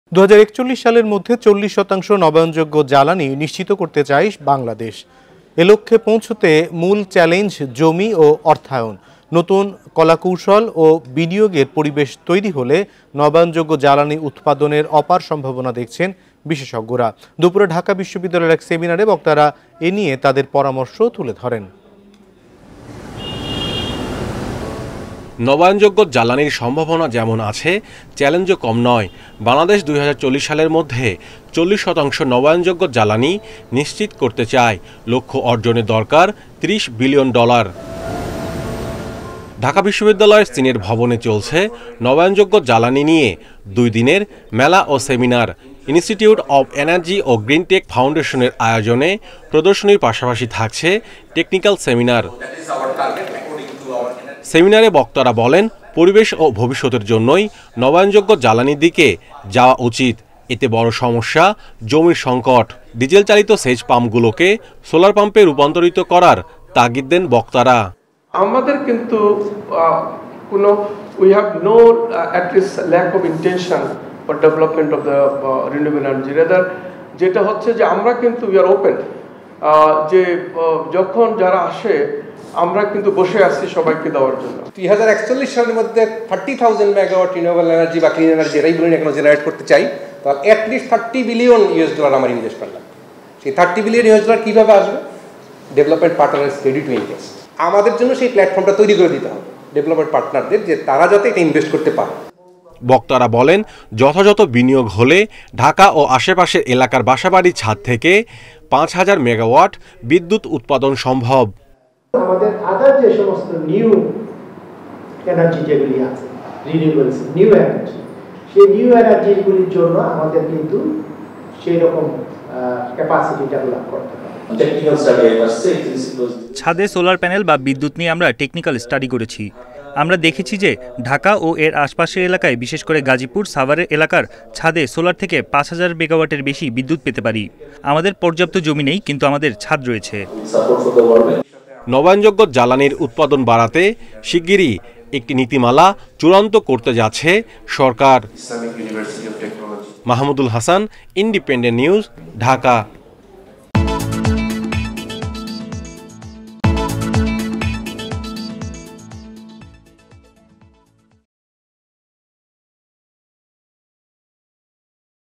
২০৪১ সালের মধ্যে ৪০ শতাংশ নবায়নযোগ্য জ্বালানি নিশ্চিত করতে চায় বাংলাদেশ || Independent TV
২০৪১ সালের মধ্যে ৪০ শতাংশ নবায়নযোগ্য জ্বালানি নিশ্চিত করতে চায় বাংলাদেশ। লক্ষ্যে পৌঁছাতে মূল চ্যালেঞ্জ জমি ও অর্থায়ন। নতুন কলাকৌশল ও বিনিয়োগের পরিবেশ তৈরি হলে নবায়নযোগ্য জ্বালানি উৎপাদনের অপার সম্ভাবনা দেখছেন বিশেষজ্ঞরা। বুধবার দুপুরে ঢাকা বিশ্ববিদ্যালয়ে এক সেমিনারে বক্তারা এ নিয়ে তাদের পরামর্শ তুলে ধরেন।